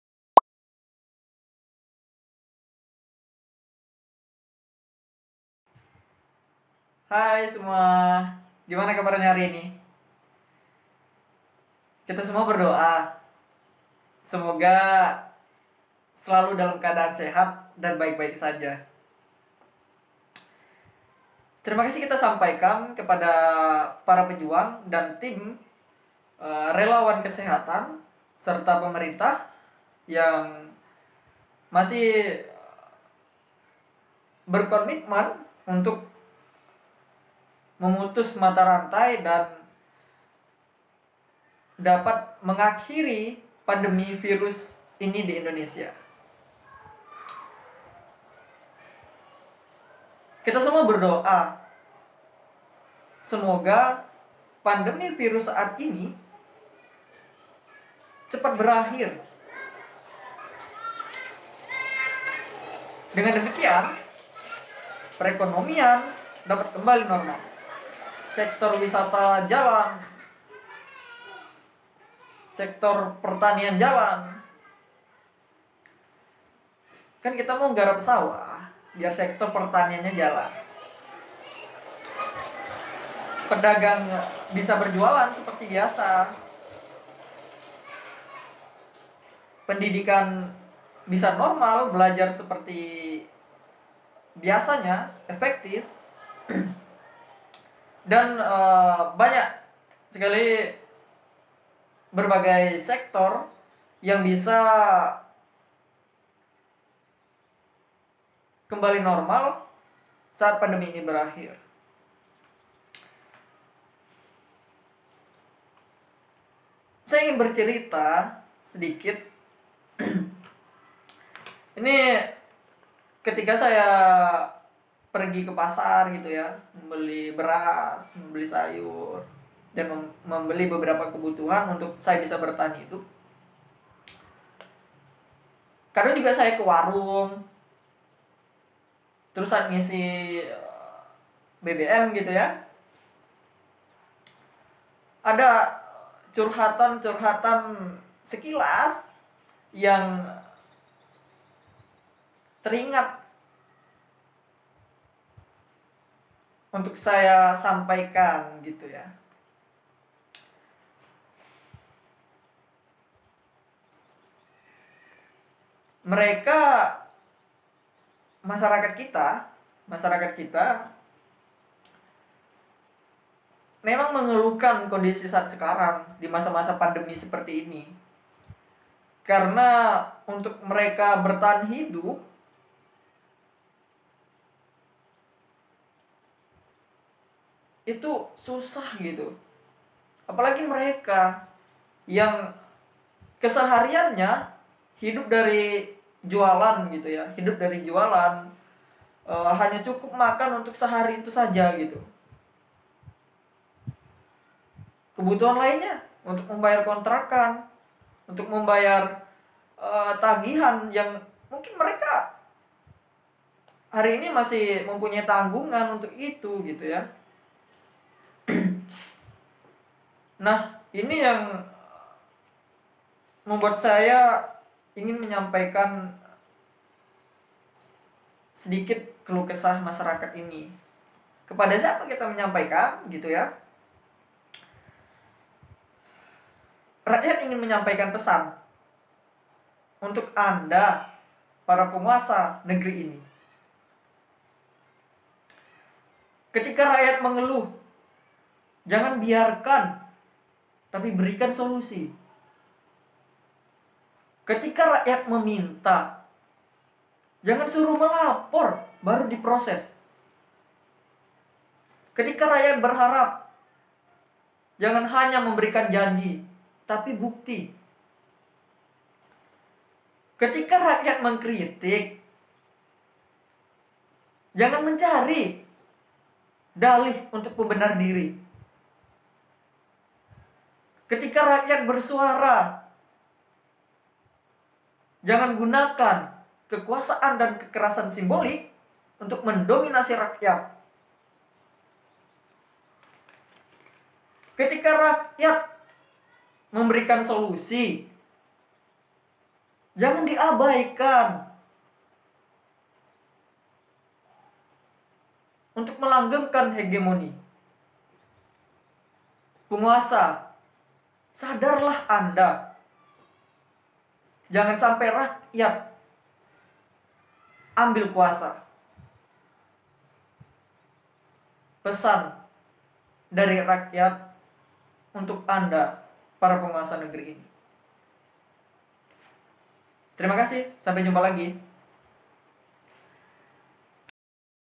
Pesan dari Rakyat untuk Anda, Para Penguasa Negeri Ini
Hai semua...gimana kabarnya hari ini ? Kita semua berdoa, semoga selalu dalam keadaan sehat dan baik baik saja.

Terimakasih, kita sampaikan kepada para pejuang dan tim relawan kesehatan, serta pemerintah yang masih berkomitmen untuk memutus mata rantai, dan dapat mengakhiri pandemi virus ini di Indonesia.

Kita semua berdoa, semoga pandemi virus saat ini cepet berakhir. Dengan demikian, perekonomian dapat kembali normal, sektor wisata jalan, sektor pertanian jalan.

Kan kita mau garap sawah, biar sektor pertaniannya jalan, pedagang bisa berjualan seperti biasa, pendidikan bisa normal, belajar seperti biasanya, efektif, dan banyak sekali berbagai sektor yang bisa kembali normal saat pandemi ini berakhir.

Saya ingin bercerita sedikit, ini ketika saya pergi ke pasar gitu ya, membeli beras, membeli sayur dan membeli beberapa kebutuhan untuk saya bisa bertahan hidup, kadang juga saya ke warung, terus saat ngisi BBM gitu ya, ada curhatan-curhatan sekilas yang teringat untuk saya sampaikan, gitu ya.

Mereka (masyarakat) kita..., memang mengeluhkan kondisi saat sekarang, dimasa-masa pandemi  seperti ini, karena untuk mereka bertahan hidup, itu susah gitu.

Apalagi mereka yang kesehariannya hidup dari jualan gitu ya, hanya cukup makan untuk sehari itu saja, gitu. Kebutuhan lainnya, untuk membayar kontrakan, untuk membayaran tagihan, yang mungkin mereka hari ini masih mempunyai tanggungan untuk itu, gitu ya.

Nah, ini yang membuat saya ingin menyampaikan, sedikit keluh kesah masyarakat ini, kepada siapa kita menyampaikan, gitu ya ?

Rakyat ingin menyampaikan pesan untuk Anda para penguasa negeri ini.

Ketika rakyat mengeluh, jangan biarkan, tapi berikan solusi

Ketika rakyat memintak, jangan suruh melapor, baru diproses

Ketika rakyat berharap, jangan hanya memberikan janji, tapi bukti

Ketika rakyat mengkritik, jangan mencari dalih untuk pembenar diri

Ketika rakyat bersuara, jangan gunakan kekuasaan dan kekerasan simbolik untuk mendominasi rakyat

Ketika rakyat memberikan solusi, jangan diabaikan, untuk melanggengkan hegemoni

Penguasa, sadarlah Anda, jangan sampai rakyat, ambil kuasa !

Terimakasih sampai jumpa lagi.

Web Blog | Kumpulan Artikel Sosiologi | YUK Baca